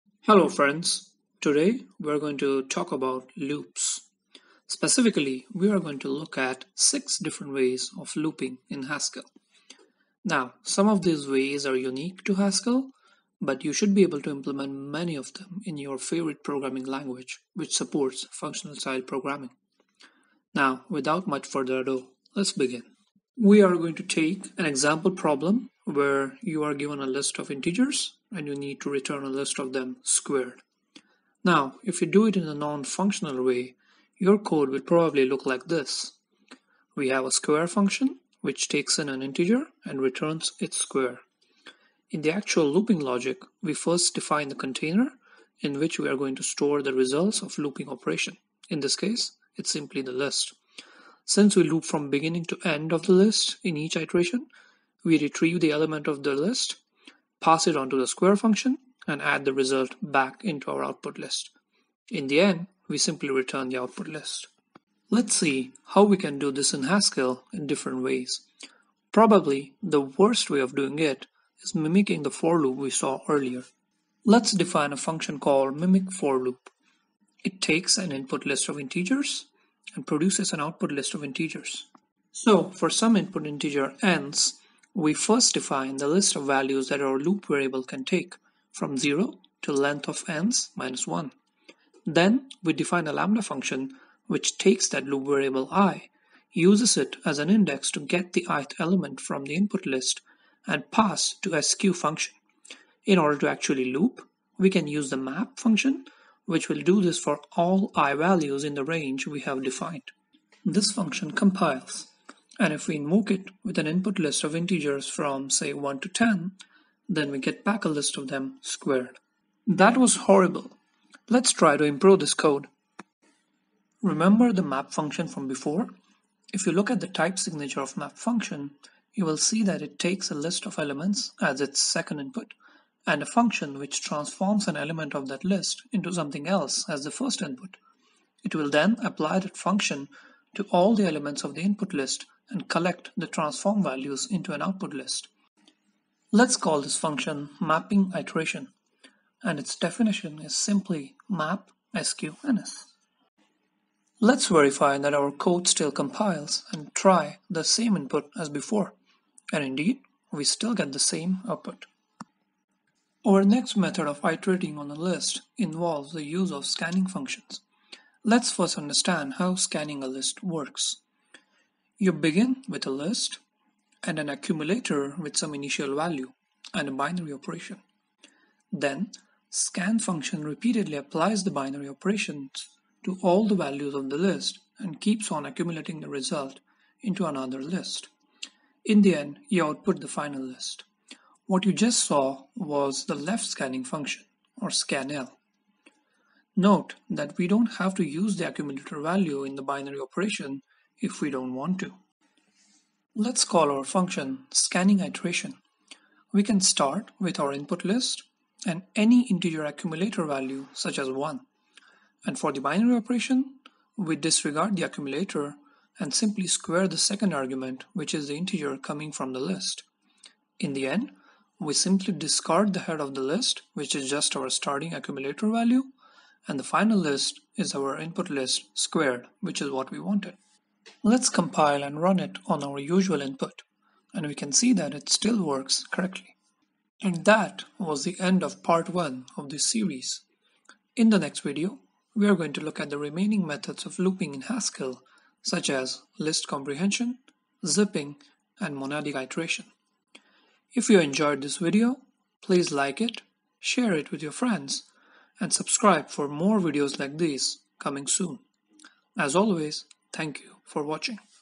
How to Loop or Iterate in Functional Programming Languages | Looping in Haskell
This video is the first of a two part series of videos. We will take a look at various ways of looping or iterating over a container like list in Haskell.

Instead of the good old fashioned for loops, we have a bunch of exciting new ways or iterating in a functional style in Haskell. In total, I am going to talk about six ways of iterating over a list, three of which are present in this video and the remaining coming up in Part 2.

The three methods I talked about are -

== Mimicking a Java style for loop - This is a bit of a contrived example, but it serves the purpose of showing the need to leave behind the 'for loop' style thinking.

== Iterating with mapping - The most common way of iterating when you want to transform a list of one type into some other type.

== Iterating with scanning functions - Haskell provides powerful abstractions like scanl and scanr functions which let you do interesting things with lists.

Coming up in next video (Part 2) -

== List comprehension - You thought only Python has it? Haskell had it way before that!

== Zipping - Zip zip zip! Protip: You can zip a list with itself! Think about it.

== Monadic Iteration - You might have heard about IO Monad in Haskell! Well, lists are Monads too! And, we can use that fact to our advantage when we want to iterate over them!